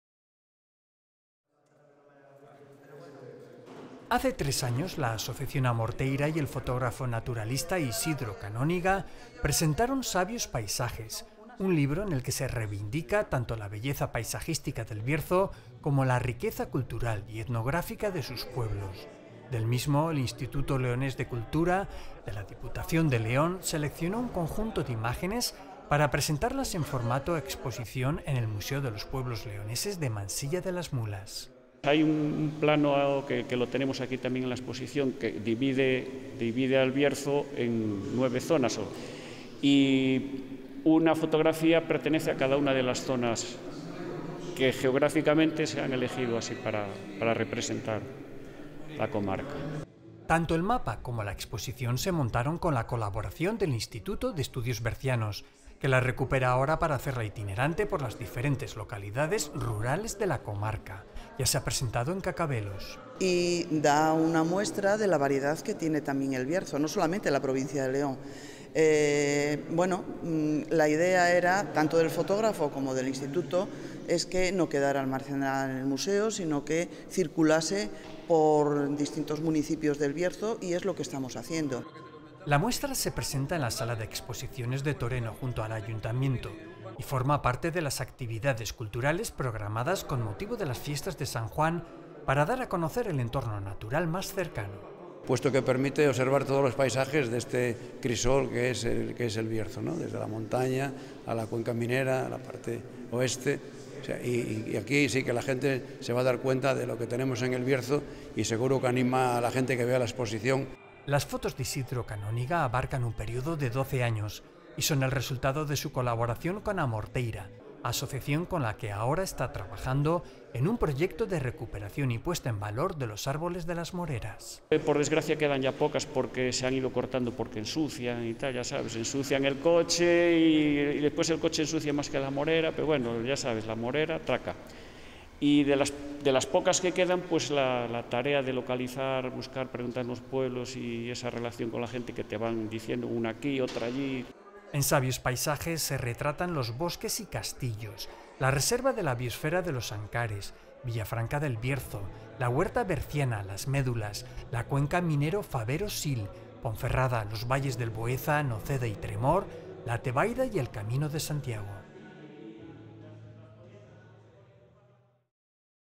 Los 'Sabios paisajes' de Isidro Canóniga llegan a Toreno
Hace tres años la asociación A Morteira y el fotógrafo naturalista, Isidro Canóniga, presentaron 'Sabios paisajes', un libro en el que se reivindica tanto la belleza paisajística del Bierzo como la riqueza cultural y etnográfica de sus pueblos. Del mismo, el Instituto Leonés de Cultura, de la Diputación de León, seleccionó un conjunto de imágenes para presentarlas en formato exposición en el Museo de los Pueblos Leoneses de Mansilla de la Mulas.

La muestra se presenta ahora en la sala de exposiciones de Toreno, junto al Ayuntamiento, y forma parte de las actividades culturales programadas con motivo de las Fiestas de San Juan para dar a conocer el entorno natural más cercano.